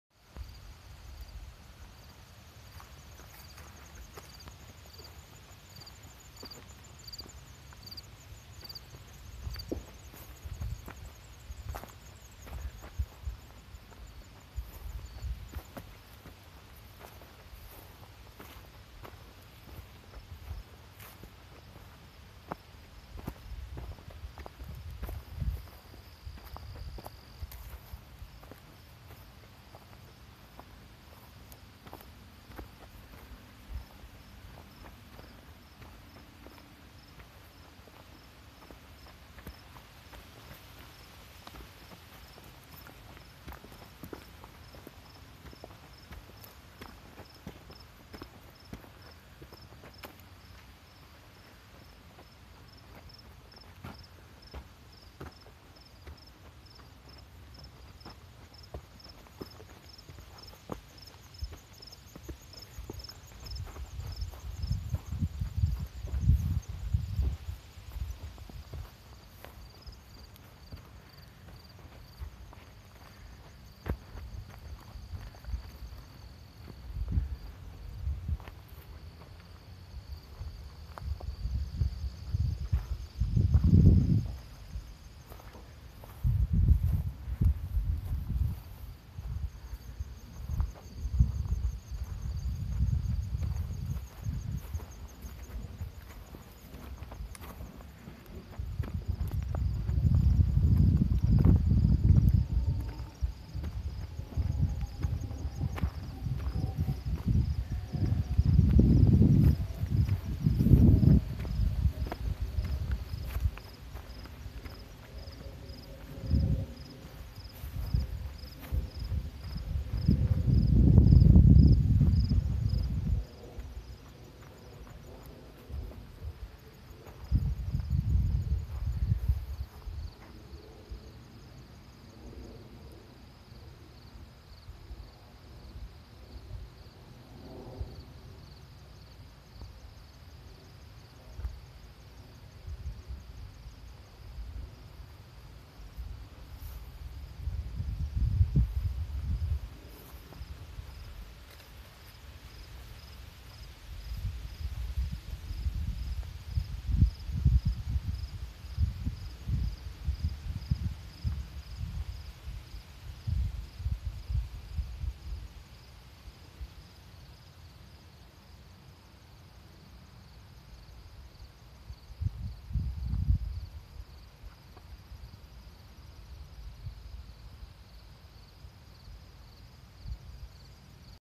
Great Hill
Reaching the summit of Great Hill. Looking northeast at the White Mountains. Not quite peak season for fall colors, nevertheless, a nice short hike on a rocky trail.